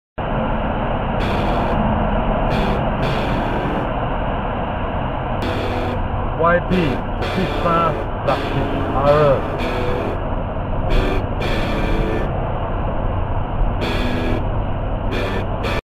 YP7367R 28Mar2019 07:37am Recklessly speeding on 2nd lane
White Mitsubishi Canter FEB21ER4SDEB YP7367R 28Mar2019 07:37am Recklessly speeding on 2nd lane at CTE Tunnel next to Cavenagh Road towards Kampong Java Flyover direction.

These action can cause serious accident to rear vehicles crashing into their rear causing chain collision and Invalid Insurance claim. Lorry Speeding on Expressway 2nd lane manner are extremely dangerous driving behaviour.